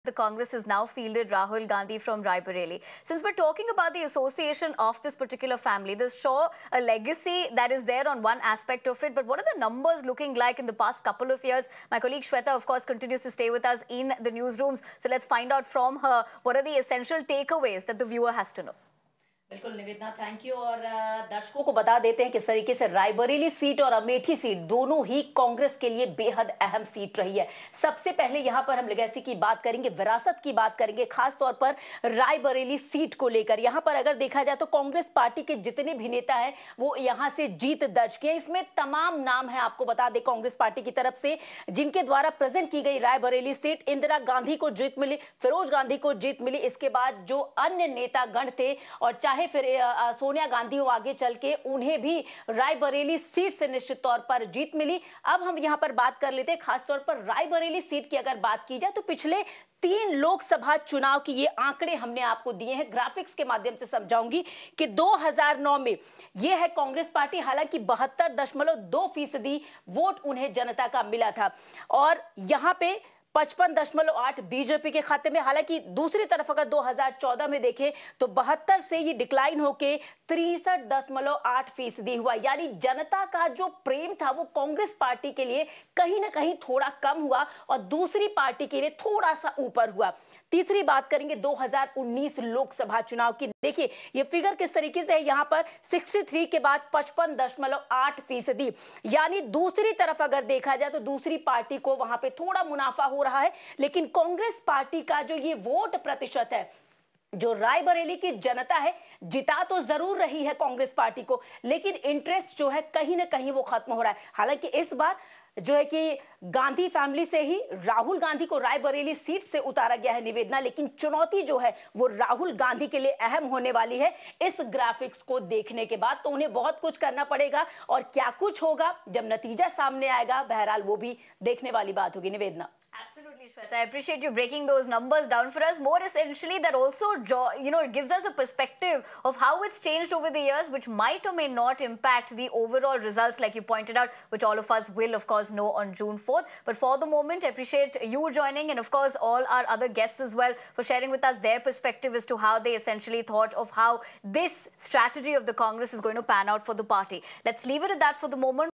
As Congress' Fortunes Decline In Raebareli Over Years, Can Rahul Gandhi Defend Legacy Bastion?
Rahul Gandhi gears up to file his nomination from Raebareli, a historic Congress bastion with a rich legacy spanning generations. This constituency, once represented by stalwarts like Indira Gandhi and Feroz Gandhi, holds immense symbolic value for the Congress party. After weeks of anticipation, Rahul Gandhi enters the fray, facing the BJP's Dinesh Pratap Singh. With polling scheduled for May 20, all eyes are on Raebareli.